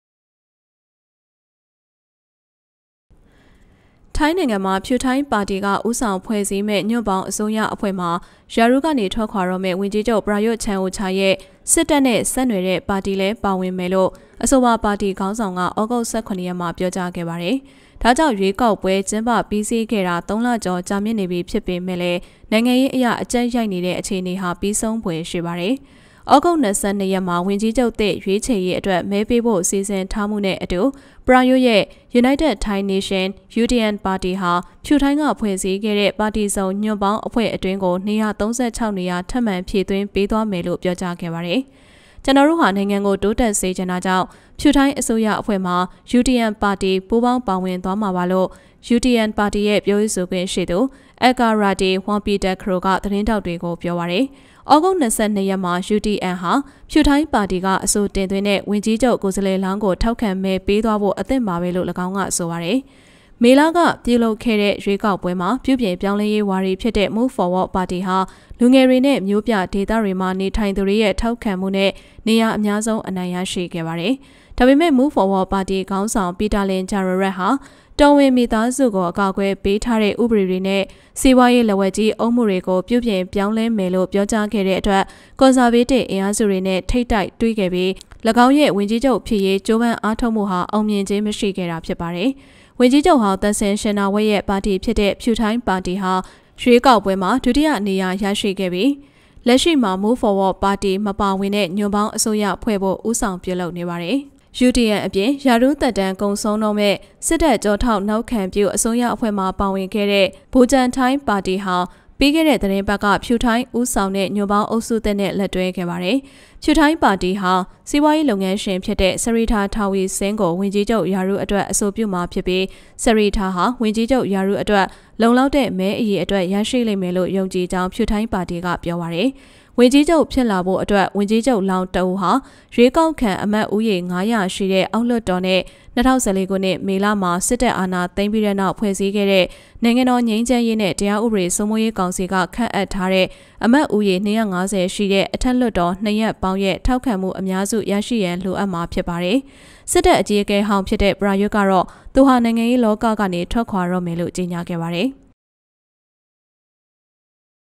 ထိုင်းညွန့်ပေါင်းအစိုးရအဖွဲ့တွင် စစ်တပ်နှင့်ဆက်နွှယ်သည့် ဝန်ကြီးချုပ်ပရာယွတ်၏ပါတီ ပါဝင်မည်
ထိုင်းနိုင်ငံမှာ ဖြူထိုင်းပါတီက ဦးဆောင်ဖွဲ့စည်းမယ့် ညွန့်ပေါင်းအစိုးရအဖွဲ့မှာ ရာထူးမှထွက်ခွာတော့မယ့် ဝန်ကြီးချုပ် ပရာယွတ်ချန်အိုချာရဲ့ စစ်တပ်နဲ့ဆက်နွှယ်တဲ့ပါတီလည်း ပါဝင်မယ်လို့ အဆိုပါပါတီခေါင်းဆောင်က သြဂုတ် ၁၇ ရက်မှာ ပြော ကြားခဲ့ပါတယ်။